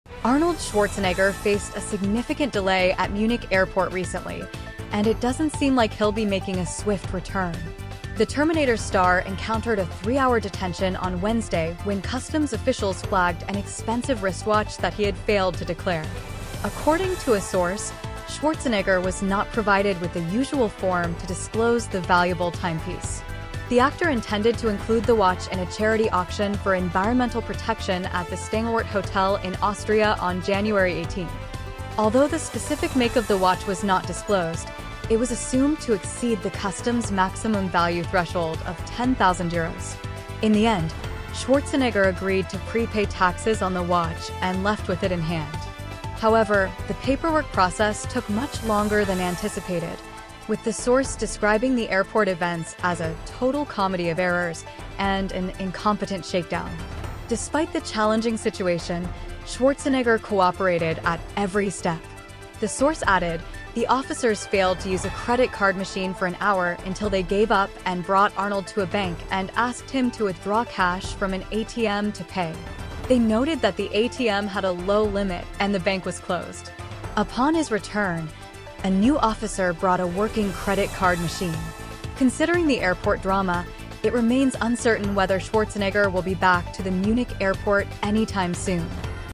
Arnold Schwarzenegger was detained at Munich airport due to an expensive watch he was carrying.
Action star Arnold Schwarzenegger faced unexpected delays at Munich airport due to a wristwatch debacle. Customs officials flagged the valuable timepiece intended for a charity auction, leading to a three-hour detention. Get the details on the airport drama, Schwarzenegger's cooperation, and the amusing twists in this "total comedy of errors." Will the Terminator be making a swift return to Munich? Find out more!